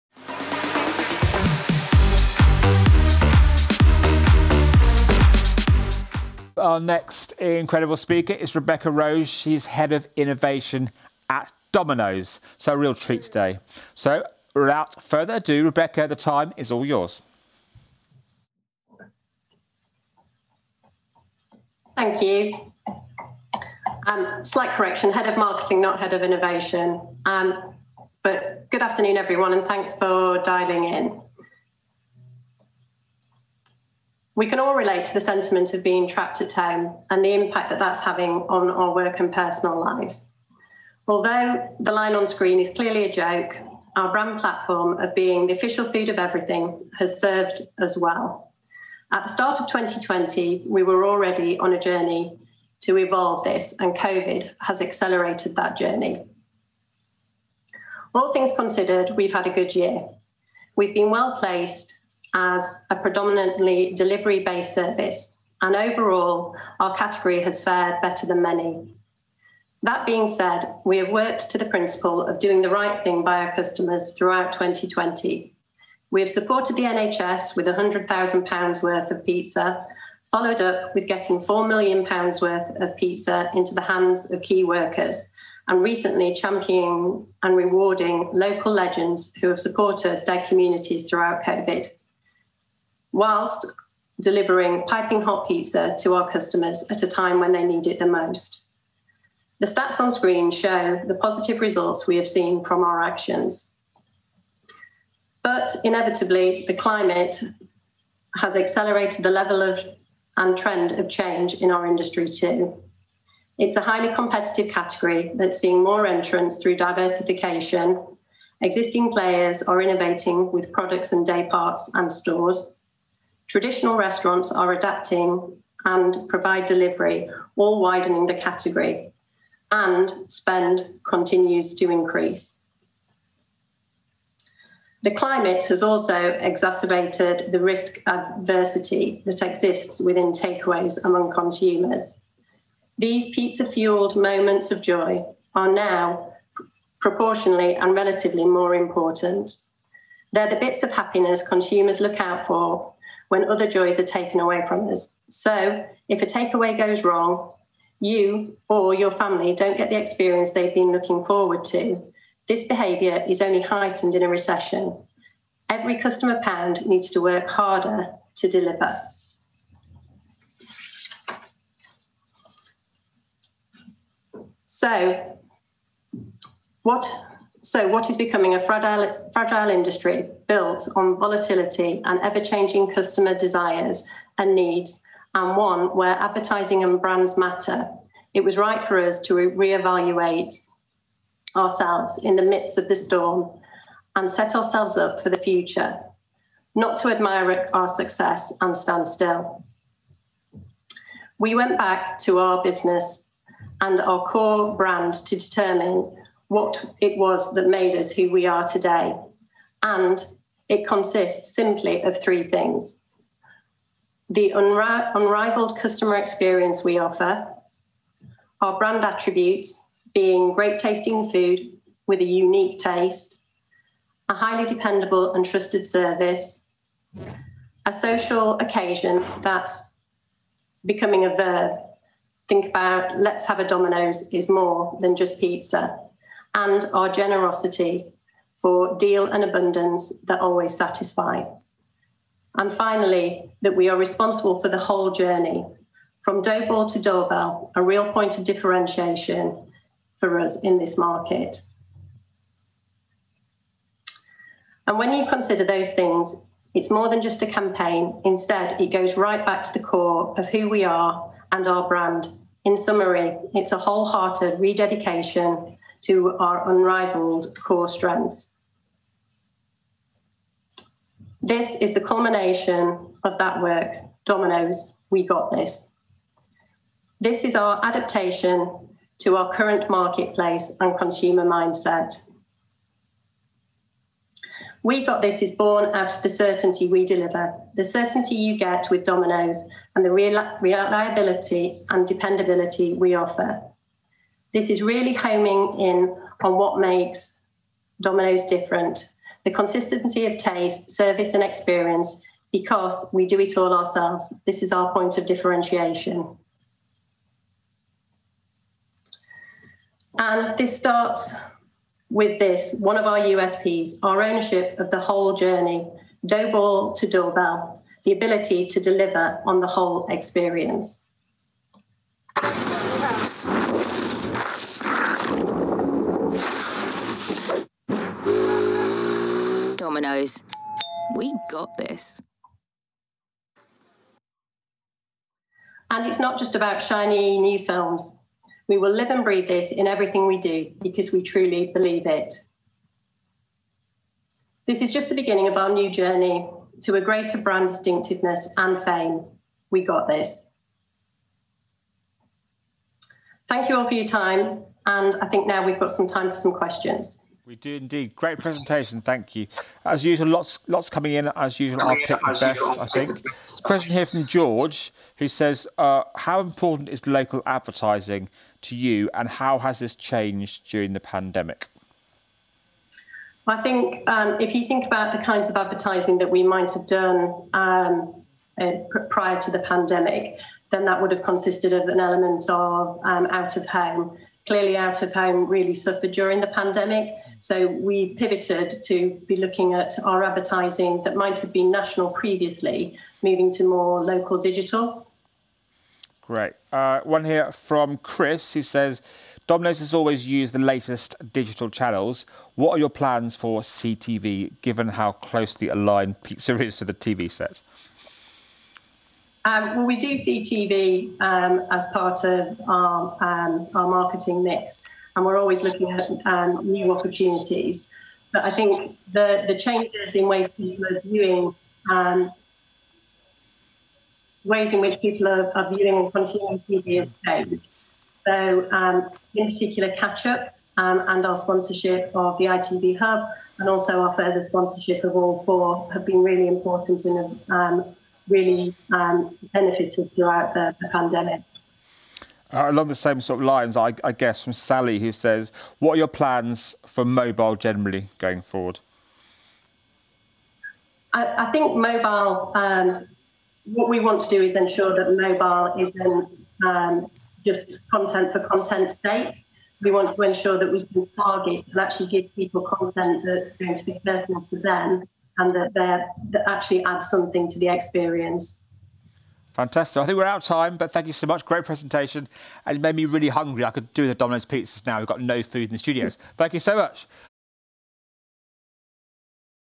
How Domino's became the "official food of lockdown" - Rebecca Rose - MAD//ANYWHERE
Domino's Head of Marketing Rebecca Rose reveals how the fast food firm tapped into the mood of the nation during the lockdown of 2020 + was able to drive sales by putting its customers first speaking at MAD//Anywhere 2020.